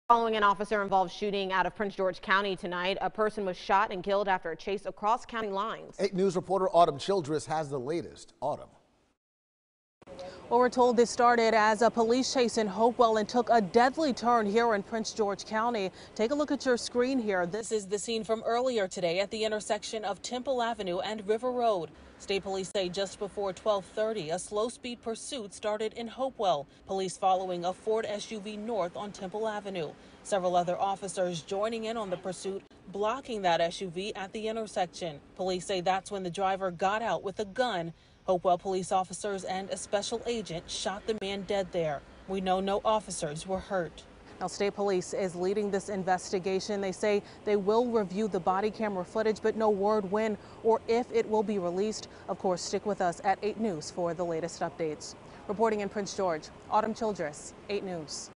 Virginia State Police investigating deadly Hopewell officer-involved shooting
Virginia State Police is investigating a fatal Hopewell officer-involved shooting that occurred just inside Prince George County, near the Hopewell line.